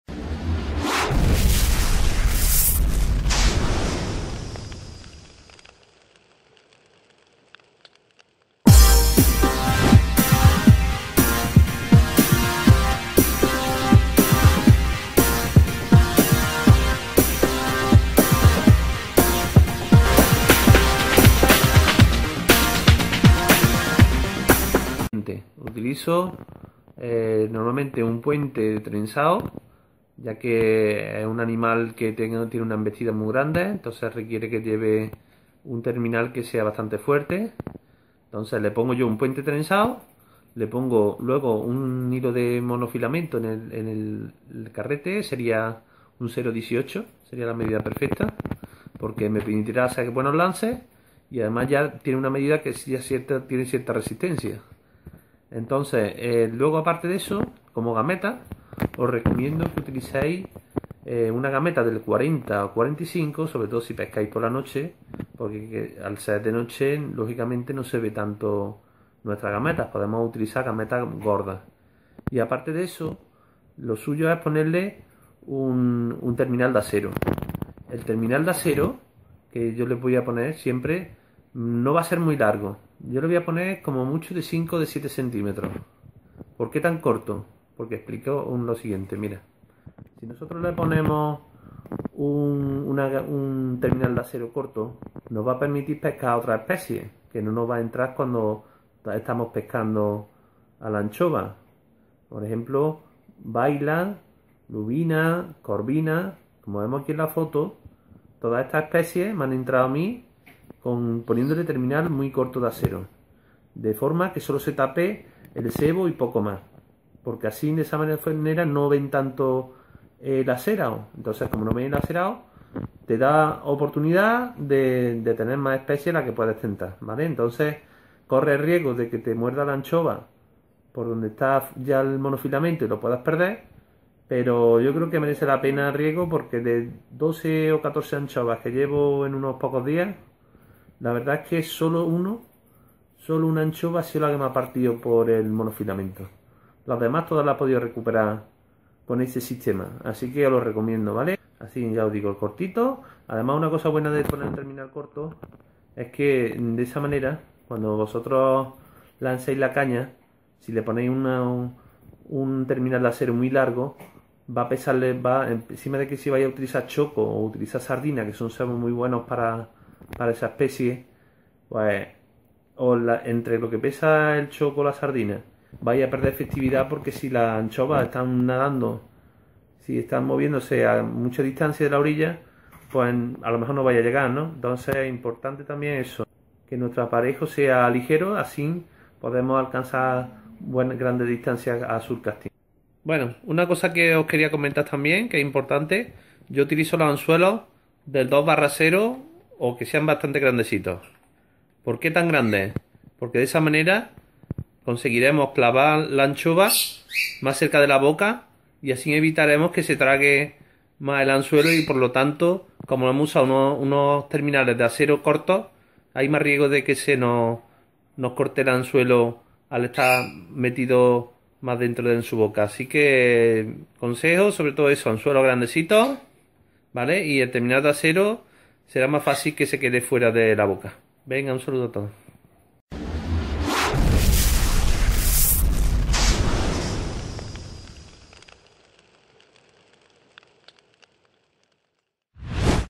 pesca surfcasting anchovas
En este tutorial de pesca a surfcasting enseño las tecnicas utilizadas para la pesca de la anchova